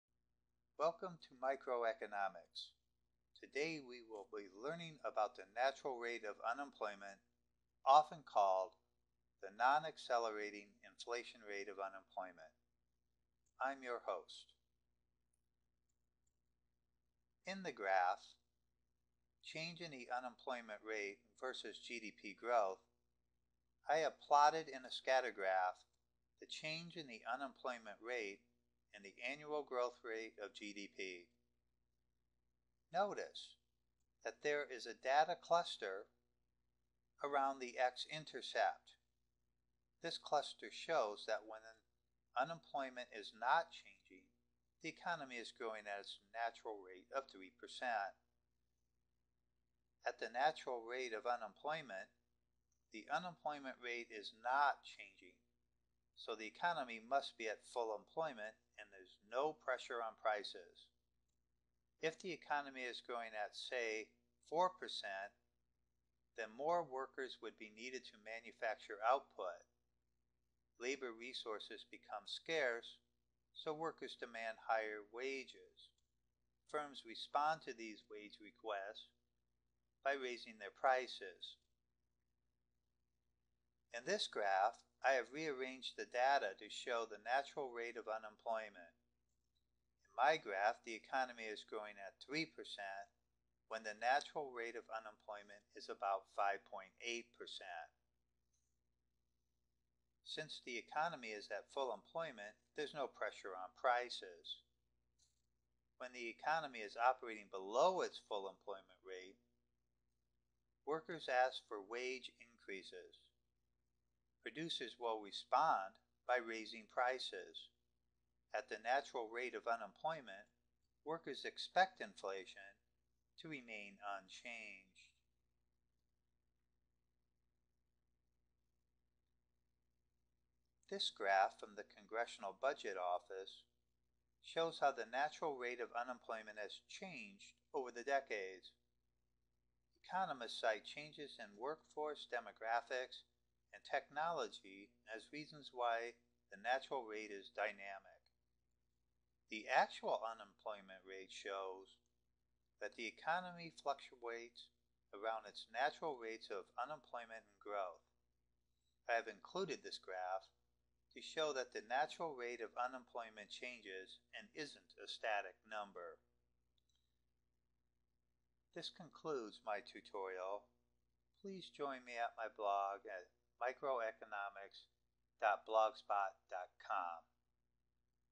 NAIRU or Natural Rate of Unemployment
This video shows scatter plots of the unemployment rate and GDP.  The plots show the Non Accelerating Inflation Rate of Unemployment.  Also included is a line graph from the Congressional Budget Office showing how the natural rate of unemployment has changed over the decades.